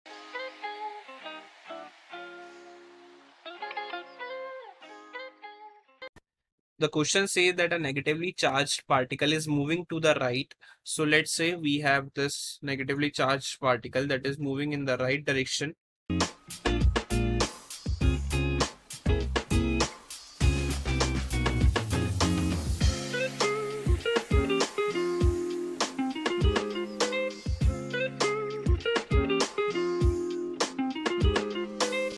A negatively charged particle is moving to the right; directly above wire having current flowing to…
A negatively charged particle is moving to the right; directly above wire having current flowing to the right as shown in the figure: In which dircction is the magnctic force exerted on thc particle? Out of the page B, Upward Downward Into the page The magnetic force i5 zero since the velocity is parallel t0 the current